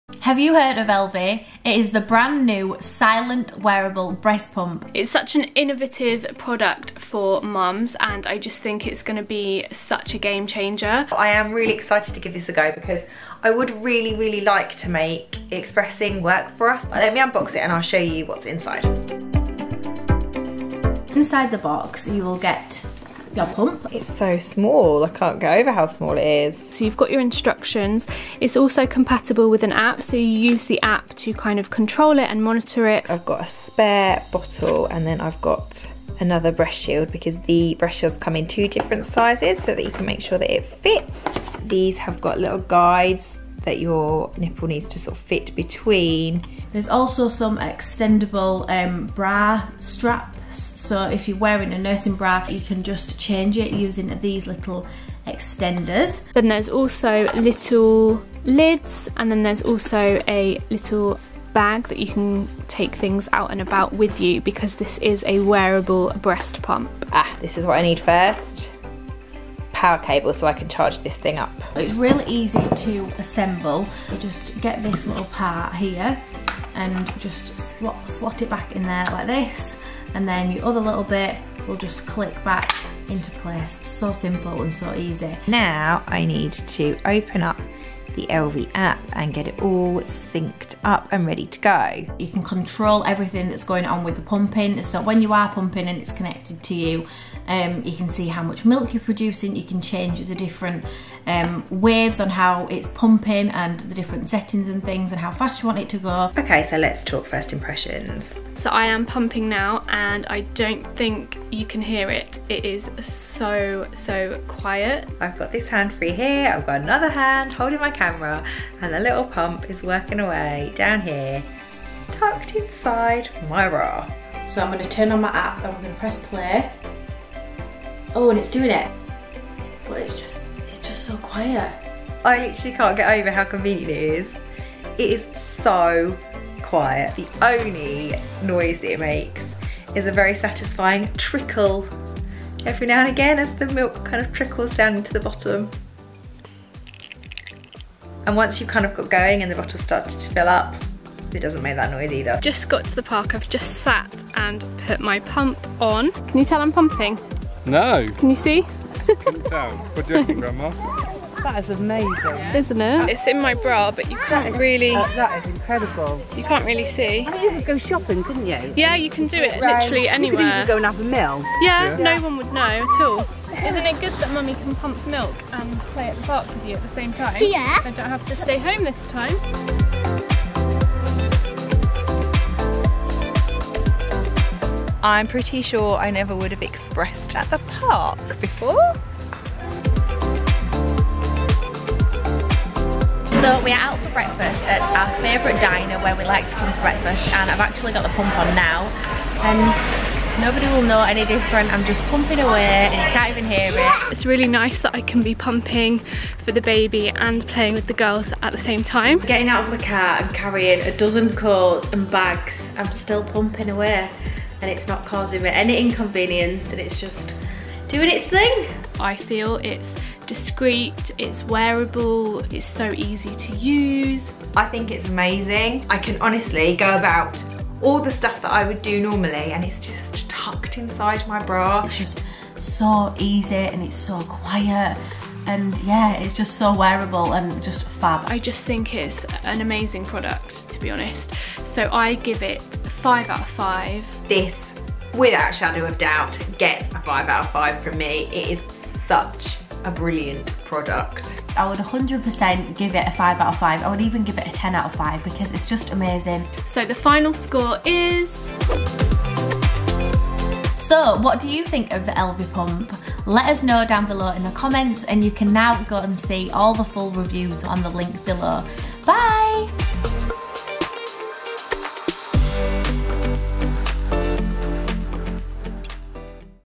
Elvie Pump: Silent & Wearable Breast Pump Review | Channel Mum | Ad
The Elvie Pump has completely revolutionised expressing breast milk! The pump is wireless and completely silent, the only sound you will hear is the milk trickling into the empty bottle and even that stops once the bottle starts to fill up. But wait, there's more... it's also wearable! It slips comfortably inside your bra, so you can pump on the go.

In this official video, we asked mums Kerry, Lucy and Rebecca to test it out and let us know how they got on!

This video contains paid for advertorial

If you want to find like-minded mums, just like you, come and join us and say hello!